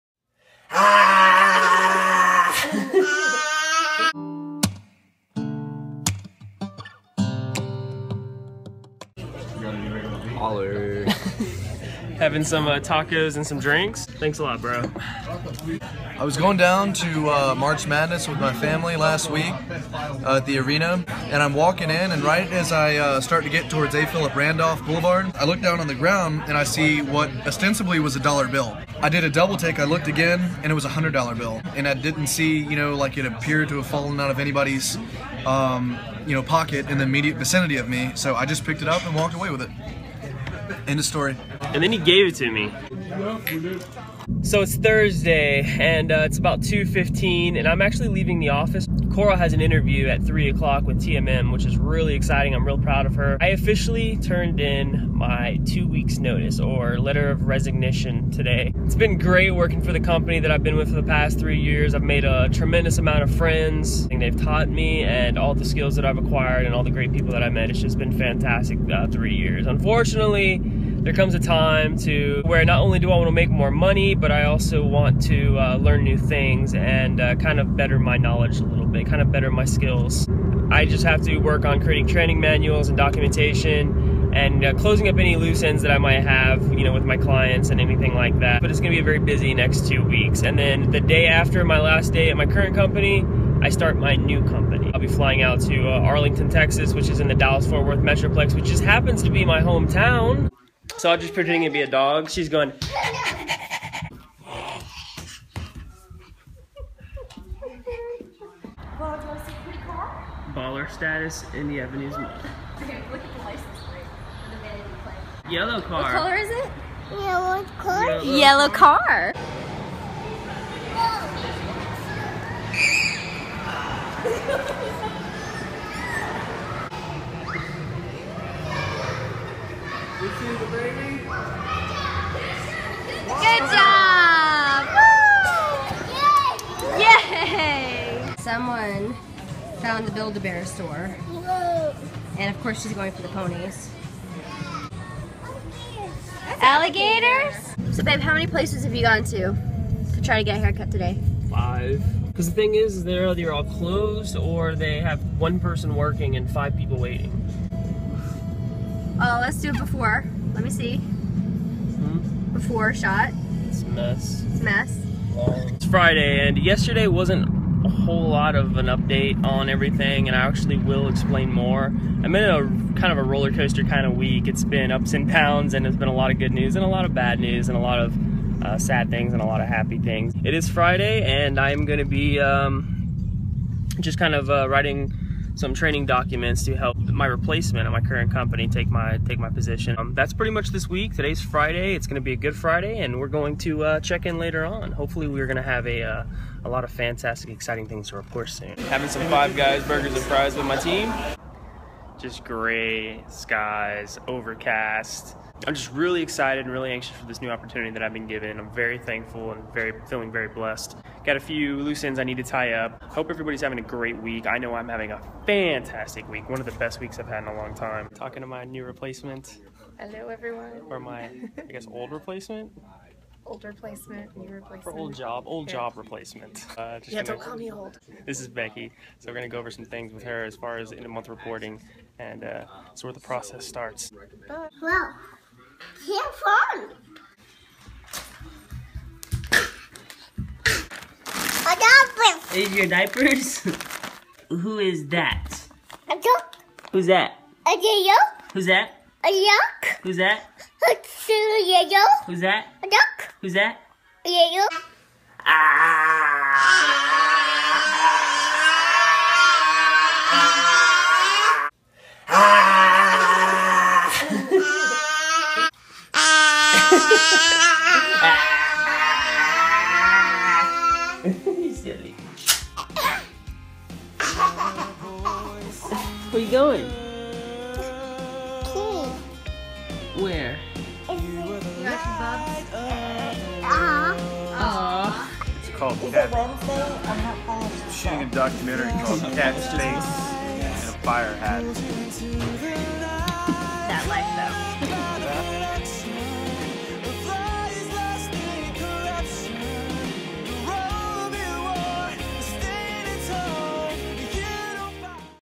DAILY VLOG - TWO WEEKS NOTICE - OMWP 36
I accepted a job offer for a great company making good money and learning some fantastic online marketing skills. So, I turned in my 2 weeks notice and letter of resignation to the company that I've spent the past 3 years with. It's bittersweet, but I'm excited for a new beginning and creating so many new memories!

All tracks used with permission.